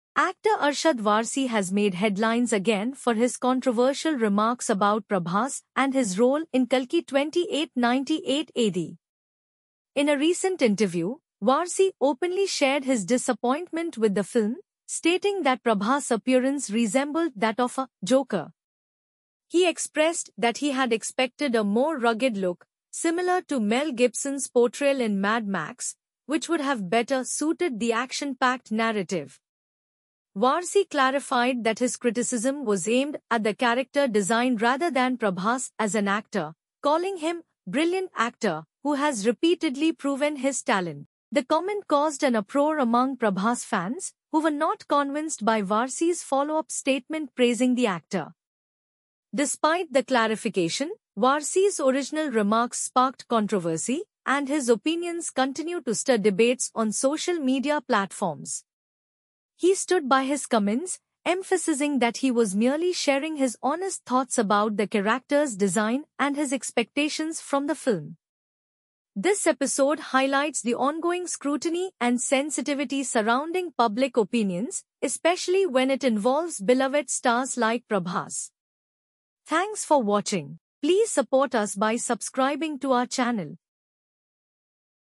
Arshad Warsi Criticizes Prabhas' Look in Kalki 2898 AD – Full Story
Arshad Warsi's comments on Prabhas' role in Kalki 2898 AD sparked controversy. Warsi criticized Prabhas' look, calling it "joker-like," but later clarified that he meant the character, not the actor. Watch the full details of the incident here!